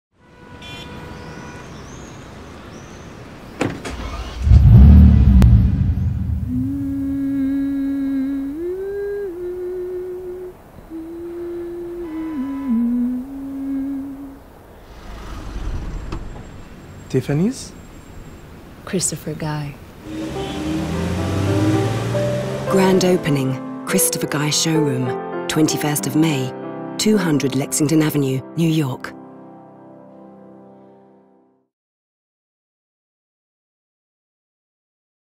Christopher Guy New York Grand Opening 2019
Grand Opening Christopher Guy Showroom
21st of May 2019
200 Lexington Ave, New York